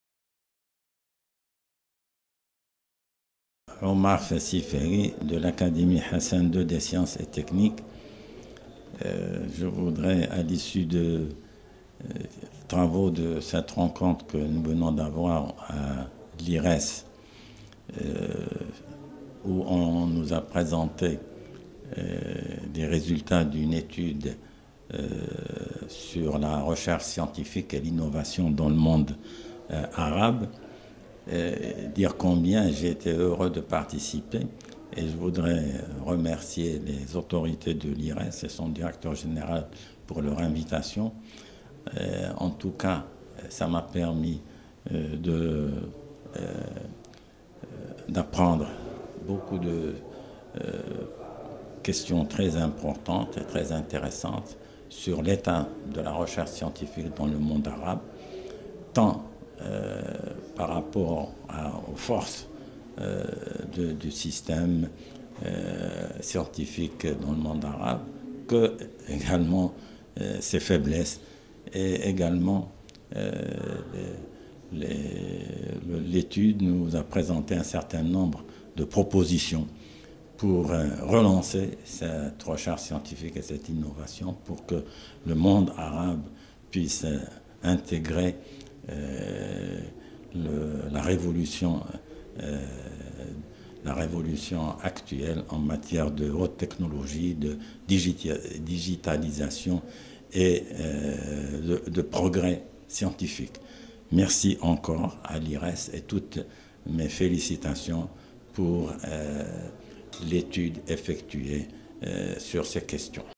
M. Omar Fassi Fihri, Secrétaire Perpétuel de l'Académie Hassan II des Sciences et Techniques
La recherche scientifique et l’innovation dans le monde arabe

L’Institut Royal des Etudes Stratégiques a organisé, le 22 mai 2019, un séminaire consacré à la discussion des conclusions préliminaires de l’étude relative à la recherche scientifique et l’innovation dans le monde arabe, en présence d’un panel comprenant de hauts responsables des Institutions publiques concernées ainsi que des experts universitaires.Cette étude a pour objet, notamment, d'examiner les réformes menées dans le monde arabe en matière de recherche scientifique et d’innovation, d'identifier les opportunités à saisir et les risques à anticiper pour renforcer le rayonnement international de cette région du monde et accélérer son développement d'ensemble et de réfléchir aux politiques publiques à mettre en place pour promouvoir la recherche scientifique et l'innovation, en se référant à quelques expériences internationales réussies en la matière.Lors de ce séminaire, l’accent a été mis sur les atouts à faire valoir et les insuffisances à combler par le monde arabe pour faire de la recherche scientifique et de l’innovation le socle de création de la richesse.